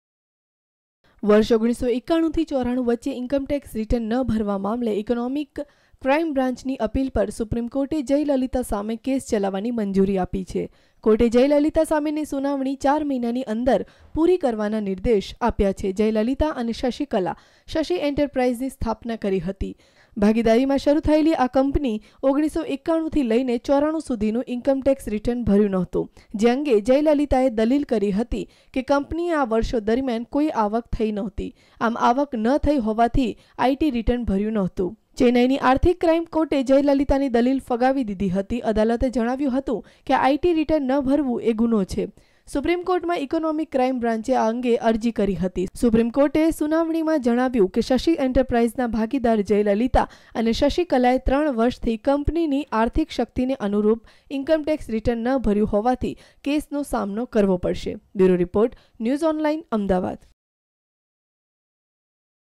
Tamilnadu CM Jaylaita in trouble
Tamilnadu CM jaylalita in trouble because incometax return.